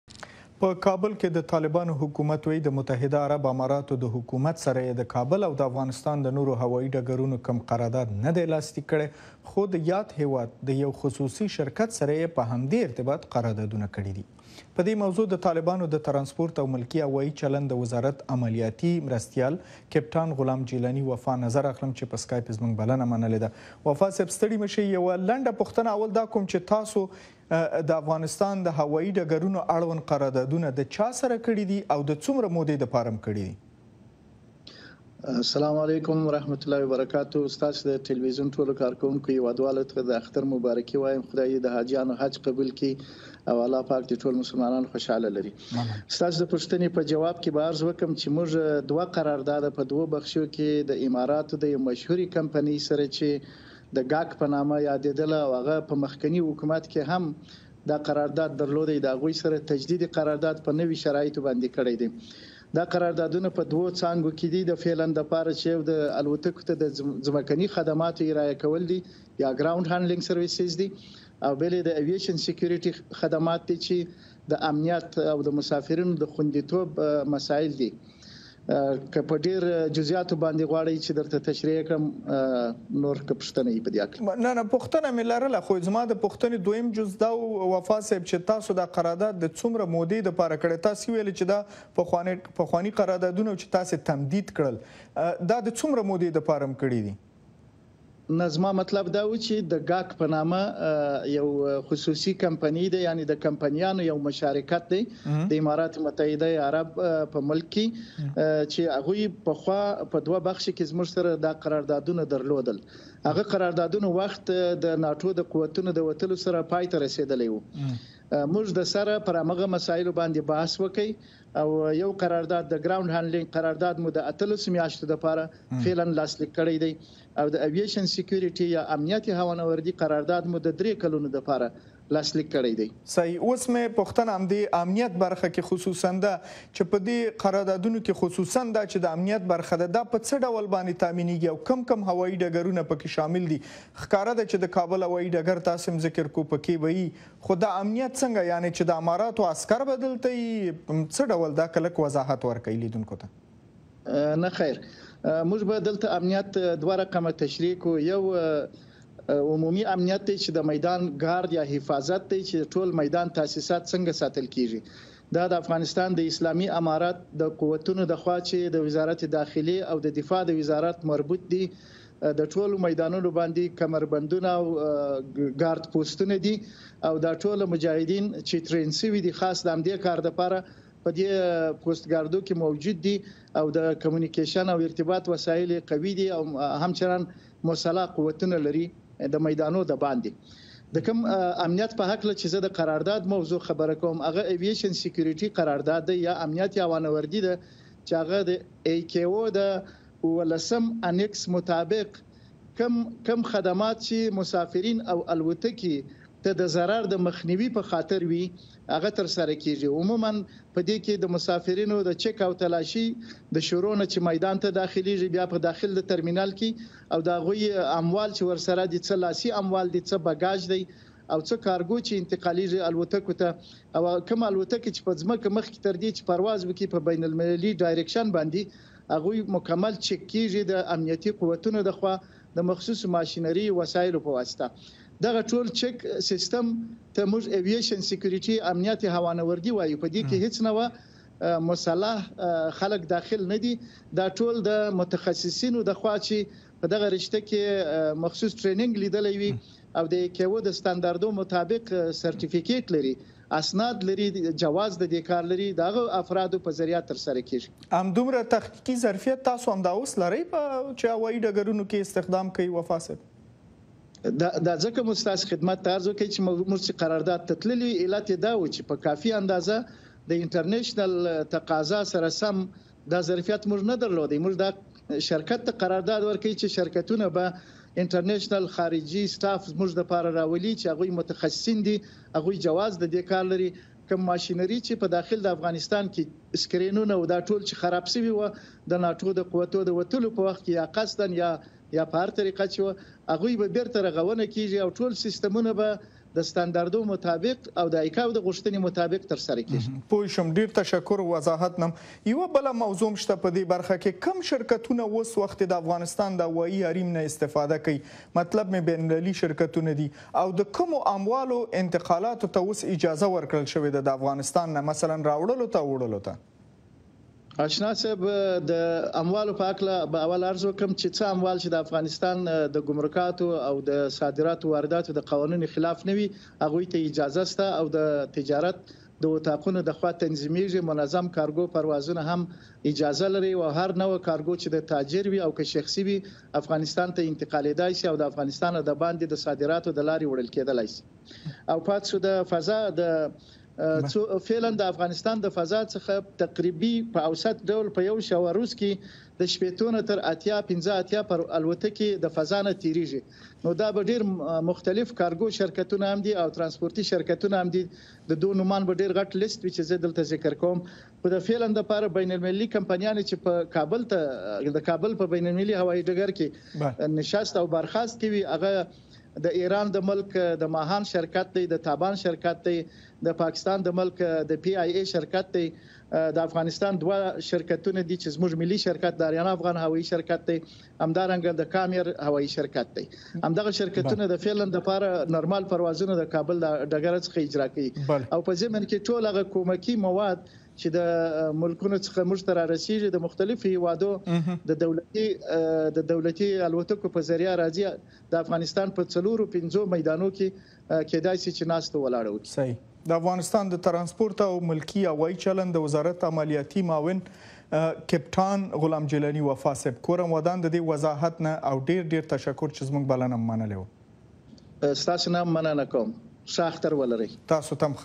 متحده عربي امارات او د افغانستان هوایي ډګرونه
د طالبانو حکومت وایي د متحده عرب اماراتو د حکومت سره يي د کابل او د افغانستان د نوروهوايي ډګرونو کوم قرارداد ندی لاسلیک کړی، خو د یادهیواد له یو خصوصي سکتور سره يی په همدې ارتباط قراردادونه کړیدي. پدې موضوع د طالبانو د ترانسپورت او ملکي هوایي چلند د وزارت عملیاتي مرستیال غلام جیلاني وفا څرګندونې دلته کتلای شئ.